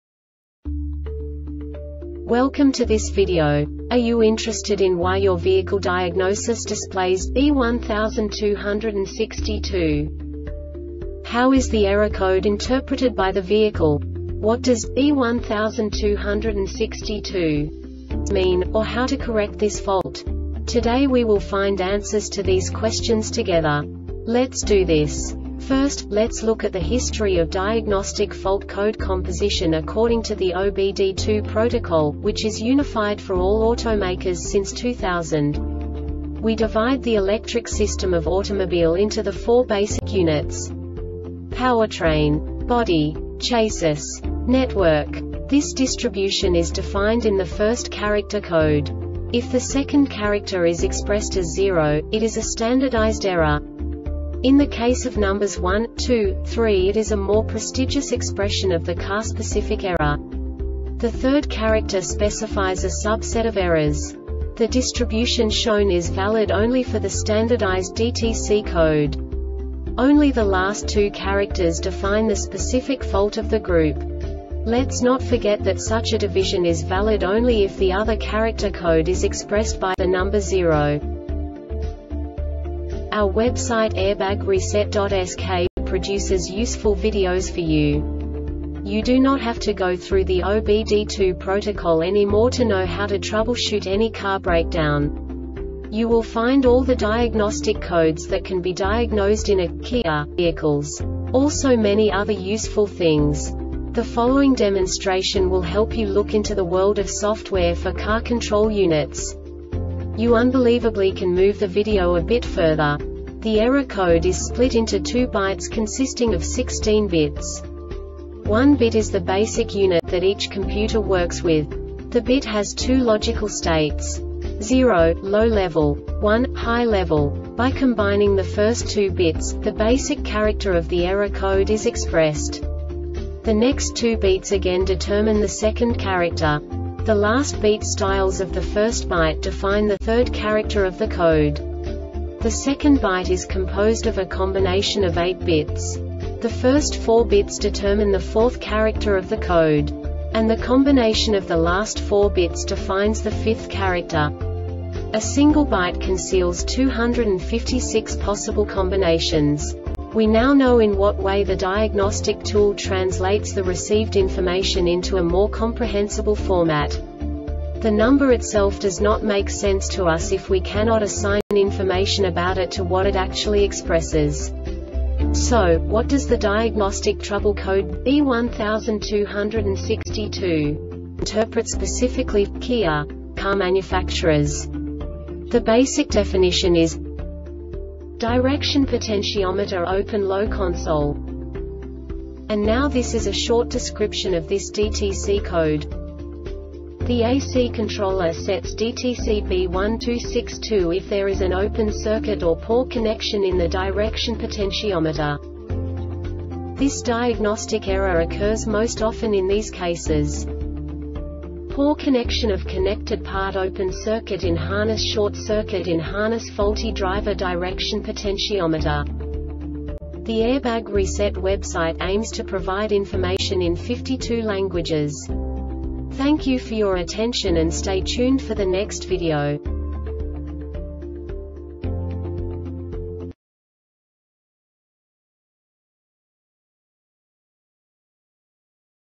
DTC KIA B1262 Short Explanation
Contents:
0:21 Basic DTC analysis according to OBD2 protocol standard.
1:48 Insight into programming
2:58 A diagnostically understandable description of the Diagnostic Trouble Code
3:05 Basic error definition

Make:
Kia
Code:
B1262
Definition:
Direction Potentiometer Open (Low)-Console
Description:
The A or C controller sets DTC B1262 if there is an open circuit or poor connection in the Direction potentiometer.
Cause:
 Poor connection of connected partOpen circuit in harnessShort circuit in harnessFaulty driver direction potentiometer